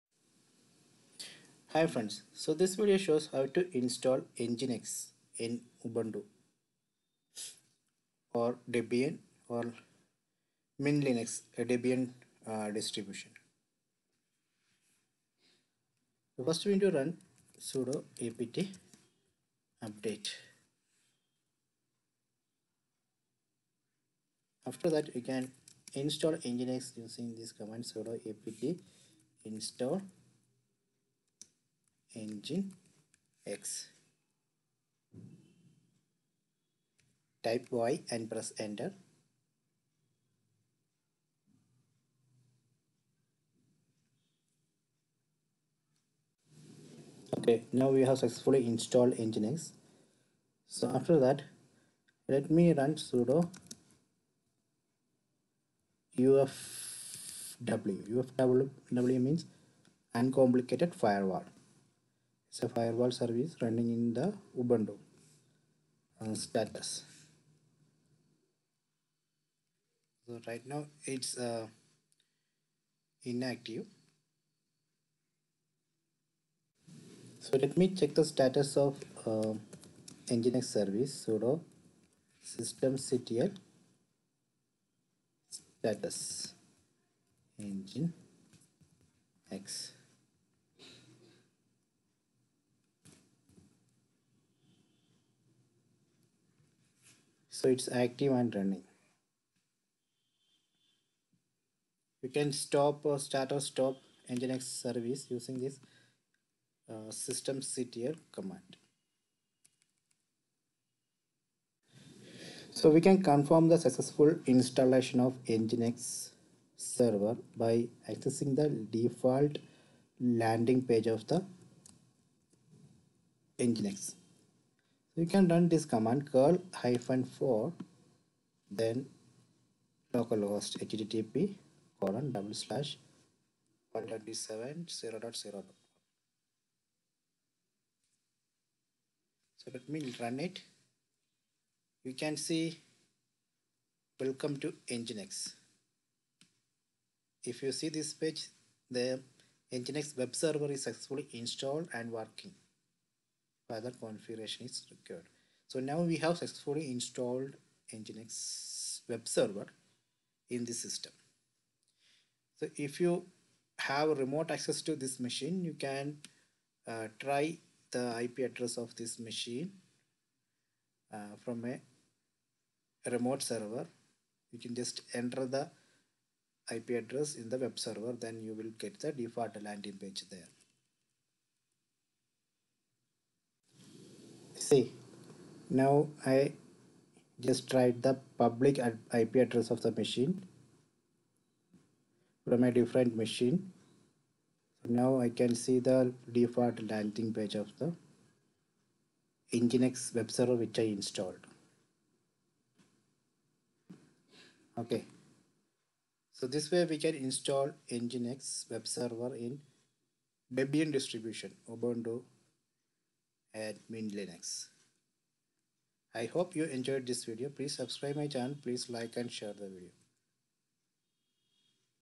How to Install Nginx Web Server in Ubuntu 19.04 18.04 Debian Linux Mint - 2019
How To Install Nginx on Ubuntu
How to Install and Configure Nginx 'Web Server' on Ubuntu
How to setup the Nginx web server on Ubuntu
Install and Configure Nginx on Ubuntu Linux
sudo apt update
sudo apt install nginx
sudo ufw status
sudo systemctl status nginx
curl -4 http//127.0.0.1